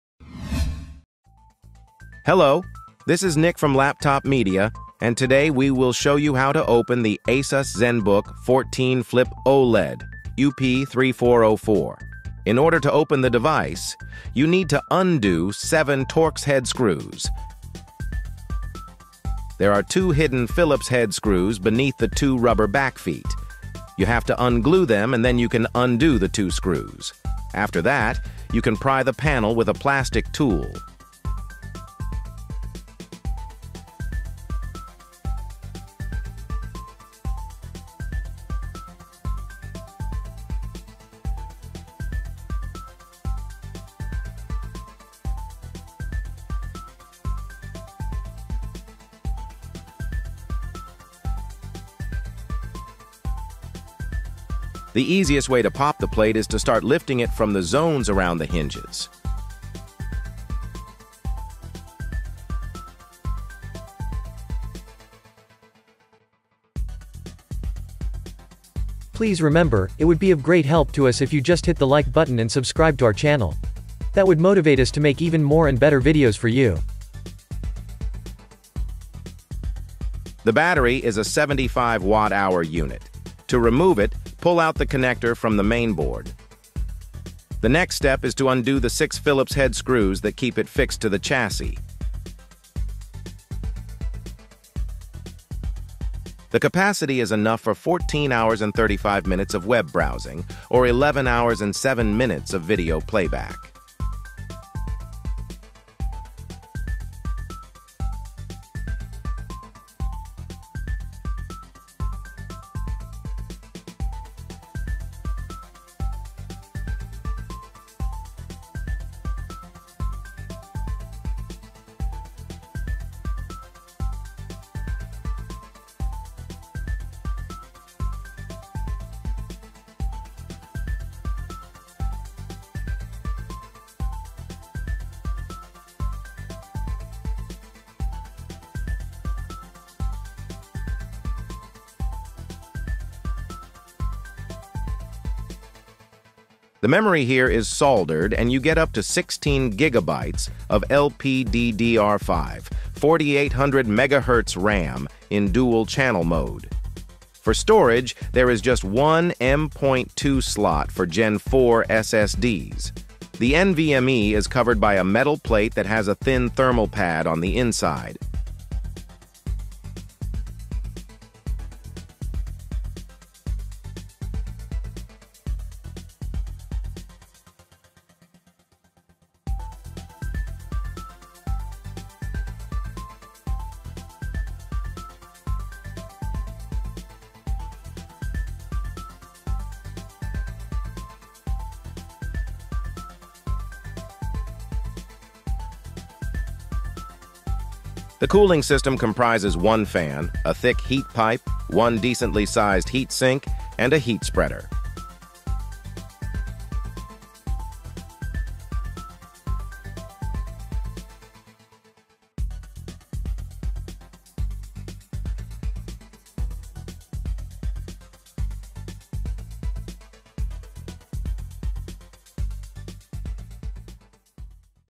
🛠️ How to open ASUS Zenbook 14 Flip OLED (UP3404) - disassembly and upgrade options
✅  LaptopMedia.com